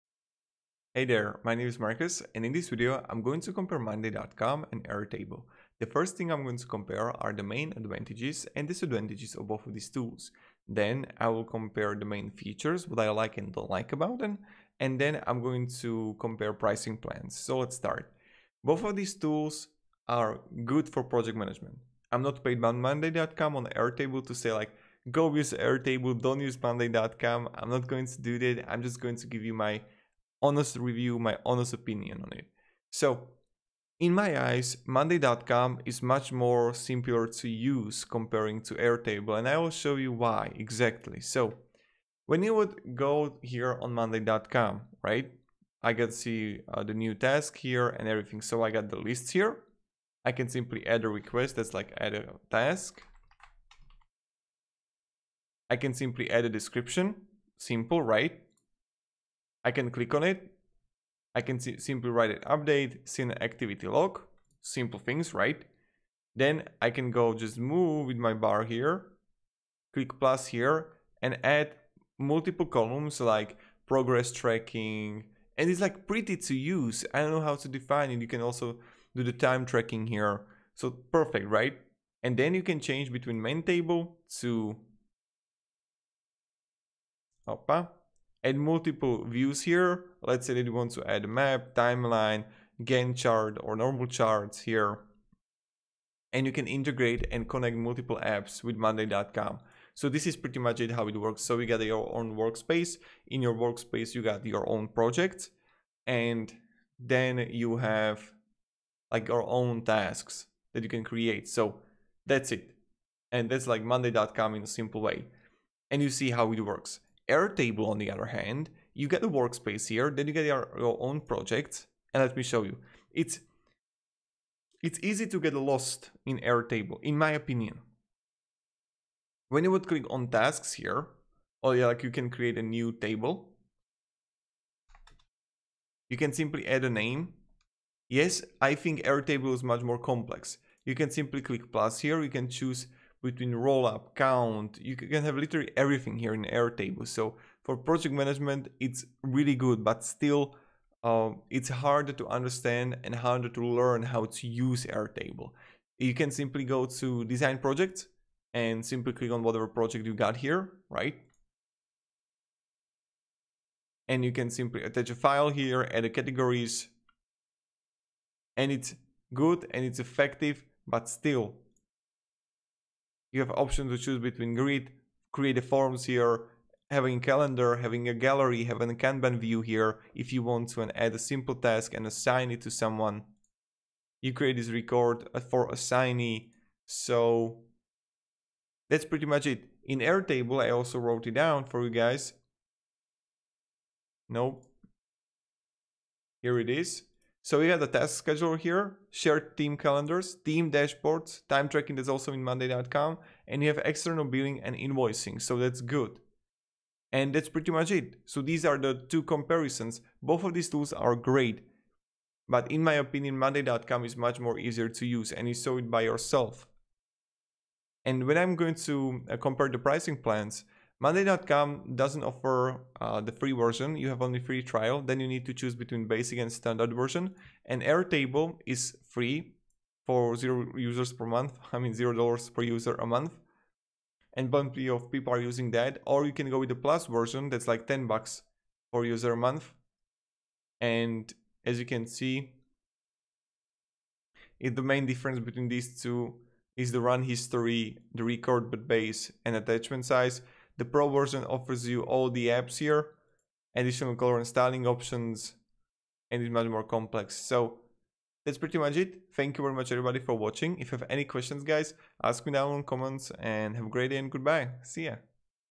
Airtable vs Monday.com - Which One Is Better?
In this video I will show you comparison between Airtable vs Monday.com

GET AMAZING FREE Tools For Your Youtube Channel To Get More Views:
Tubebuddy (For GROWTH on Youtube):